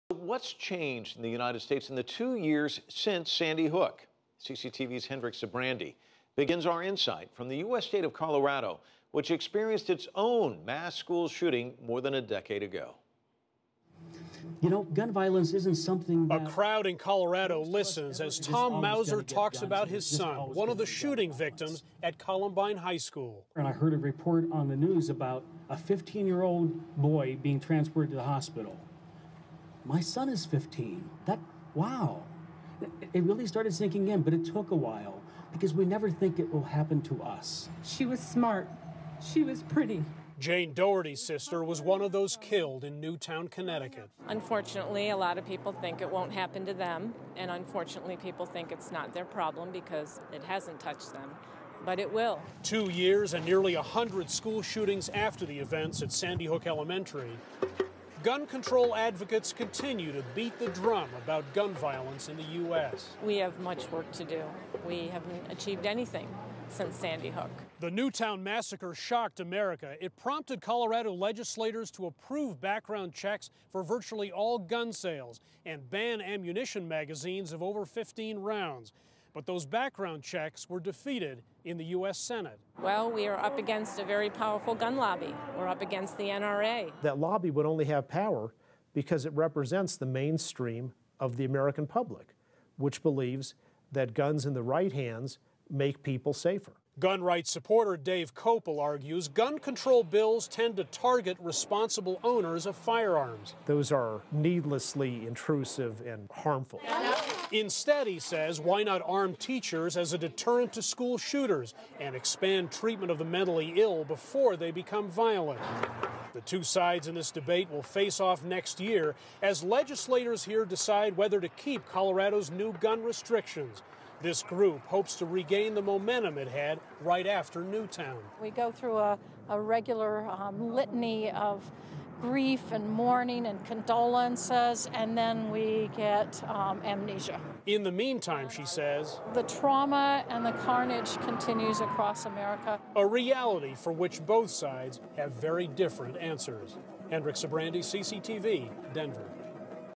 What the gun debate in America looks like two years after Sandy Hook
It's been just over two years since 20 children and six adults were killed by a lone gunman in Newtown, Connecticut. The mass shooting at Sandy Hook Elementary School sparked outrage in the U.S., and a debate about gun violence continued.The Newtown massacre prompted Colorado legislators to approve background checks for virtually all gun sales and ban large capacity ammunition magazines of more than 15 rounds, but the expanded background checks were defeated in the U.S. Senate. CCTV America's Hendrik Sybrandy reported from Denver, Colorado about where that debate stands two years after Newtown.